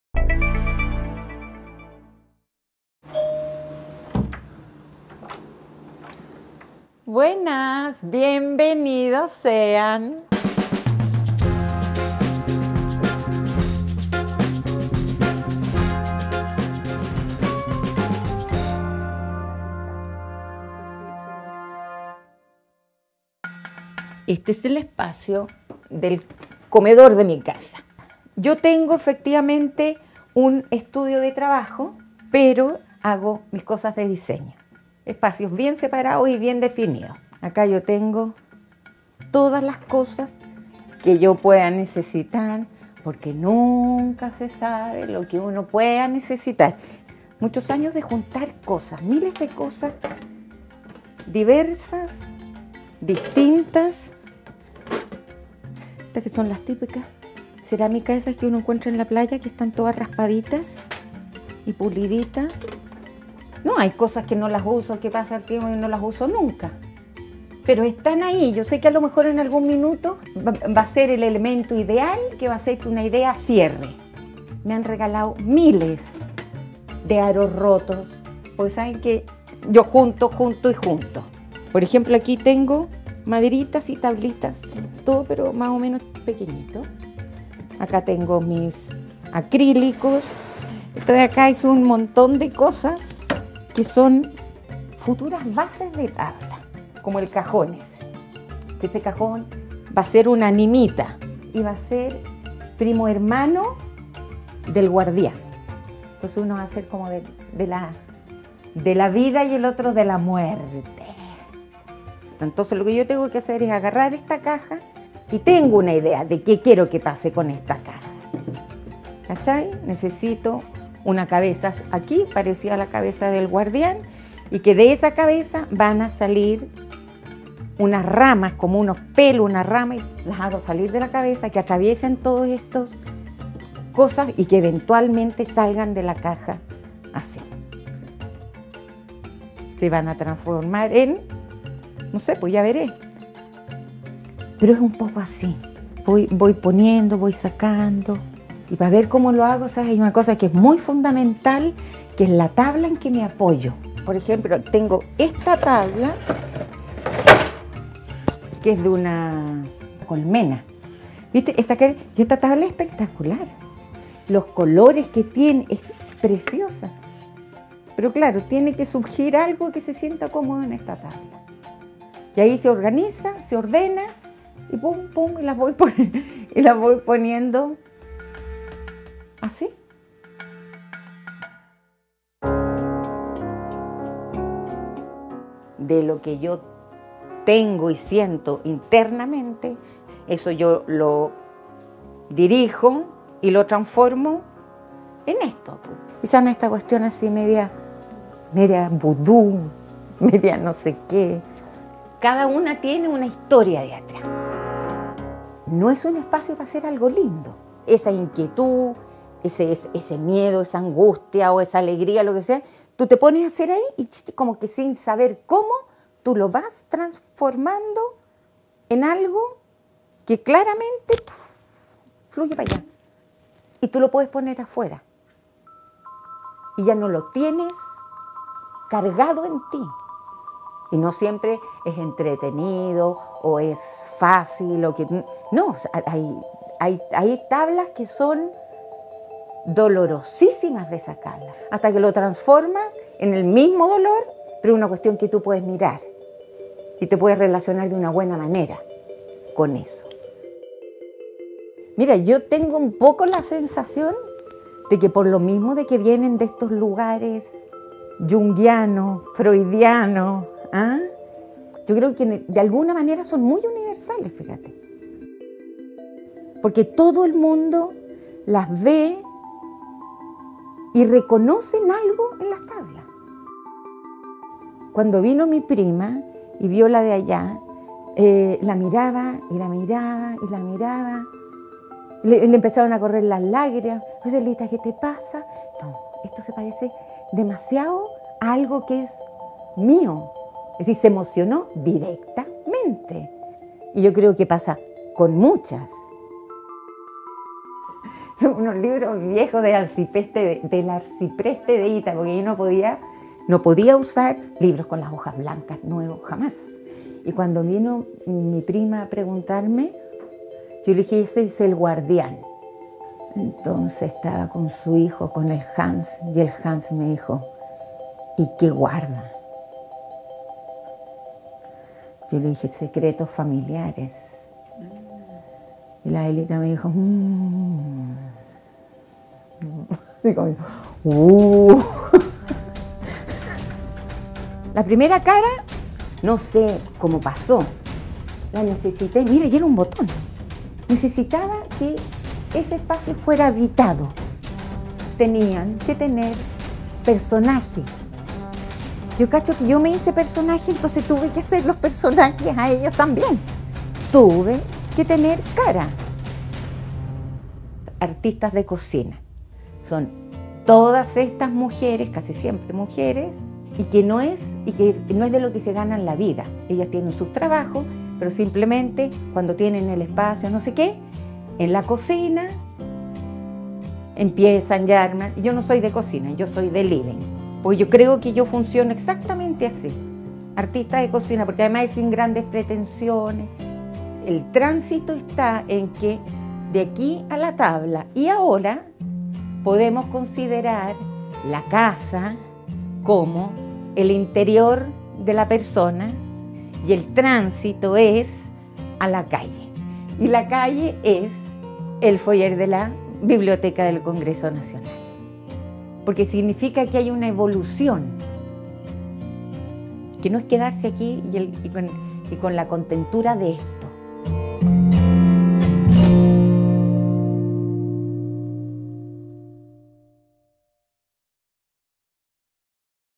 Artista Francisca Galilea presenta su trabajo previo a exponer en la BCN
La muestra "Arqueología personal", de carácter biográfico, es la suma de tiempos, lugares, experiencias, encuentros y desencuentros... Es el resultado de un largo trabajo creativo, de construir el propio lenguaje expresivo y de las pulsaciones que se transforman en obra...

Objetos encontrados en distintos lugares que se resignifican y cobran nueva vida dotándolos de inquietante simbolismo. Entonces emergen del caos estas tablas. Balsas armadas con pedazos de madera vieja, de desecho, quemados por el sol, pulidos por el mar y la arena...

La muestra estará disponible durante julio en la sede de la Biblioteca del Congreso Nacional de Chile, en calle Compañía 1175, Santiago.